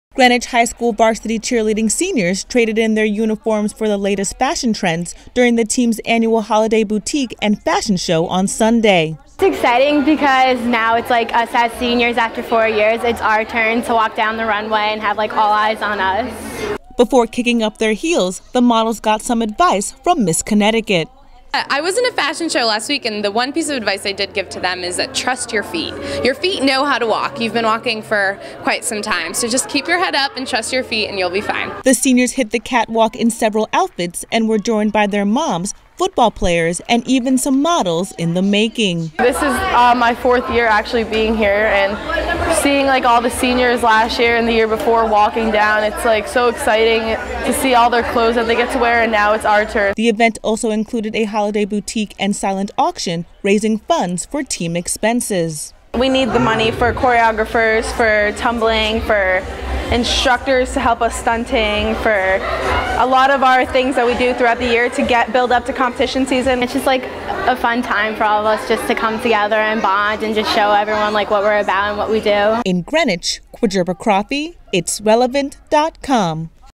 Models Full of Spirit Walk The GHS Catwalk
Greenwich High School Varsity Cheerleading seniors traded in their uniforms for the latest fashion trends during the teams annual Holiday Boutique and Fashion Show on Sunday.

Senior and Greenwich High School Varsity Cheerleading Captain Amanda Rosabella said, "It's exciting because now it's us as seniors, after 4 years it's our turn to walk down the runway and have all eyes on us."
Before kicking up their heels, the models got some advice from Miss Connecticut.

Miss Connecticut 2013 Kaitlyn Tarpey said, "I was in a fashion show last week and the one piece of advice I did give to them is "trust your feet." Your feet know how to walk. You've been walking for quite some time so just keep your head up, trust your feet and you'll be fine."
The seniors hit the catwalk in several outfits and were joined by their moms, football players and even some models in the making.

Senior and Greenwich High School Varsity Cheerleading Captain Ashley Grudzinski said, "This is my fourth year actually being here and seeing all the seniors last year and the year before walking down, it's so exciting to see all the clothes they get to wear and now it's our turn."

The event also included a holiday boutique and silent auction, raising funds for team expenses.

Senior and Greewich High School Varsity Cheerleading Captain TaylorIngraham said, "We need the money for choreographers, for tumbling, for instructors to help us with stunting and for a lot of our things that we do throughout the year to get us ready and build up for competition season."

Rosabella added, "It's a fun time for all of us to come together and bond and show everyone what we're about and what we do."